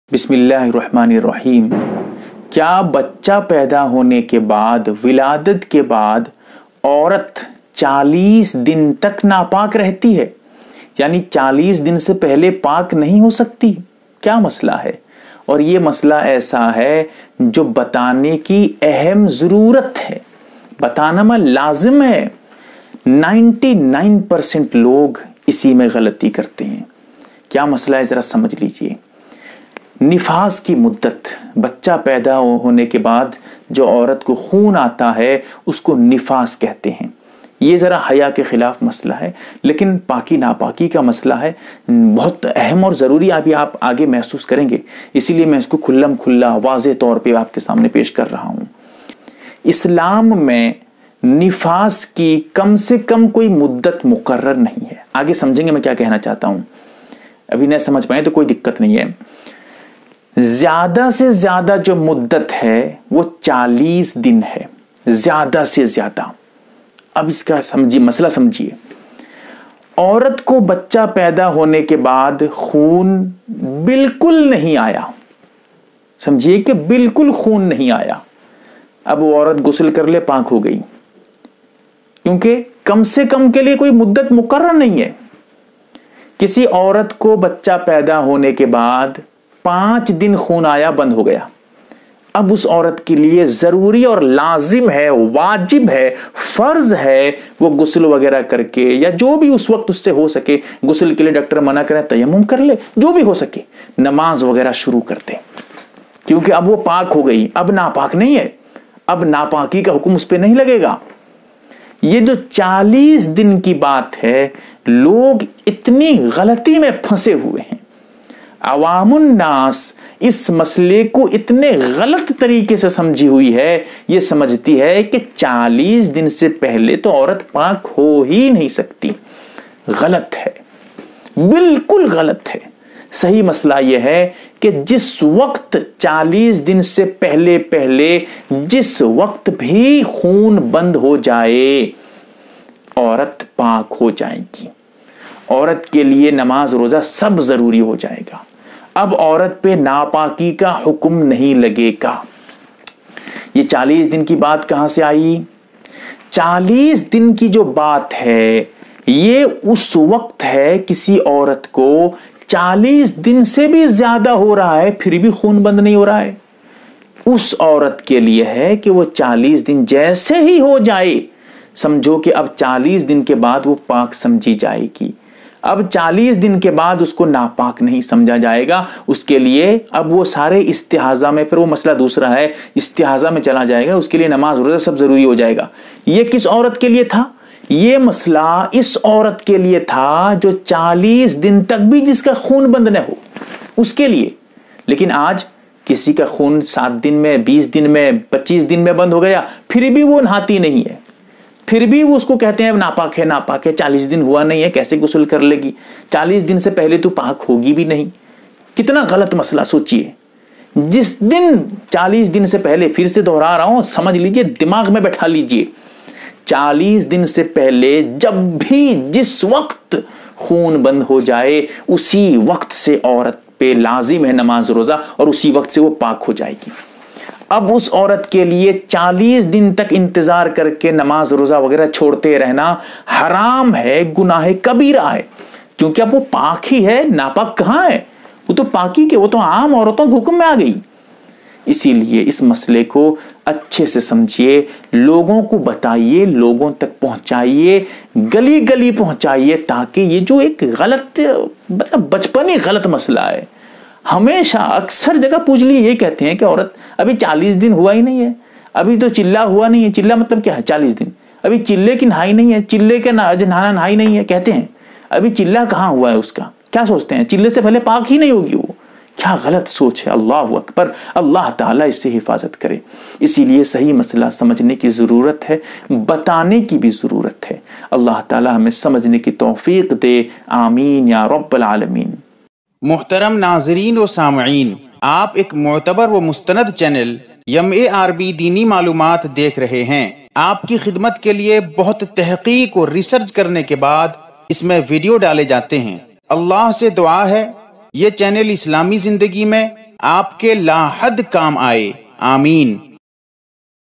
Kya Bacha Hone Ke Bad Ladies 40 Din Napak Hoti Hai | (Delivery)New Baby Hone Ke Bad Napaki Ke Masail
Bacha paida hone ke bad aurat kitne din tak naapak hoti hai? kya delivery ke bad 40 din napaki hoti hai full details masail ka bayan by Mufti Abdur Rahman Banahara MARB Deeni Malumaat YouTube Channel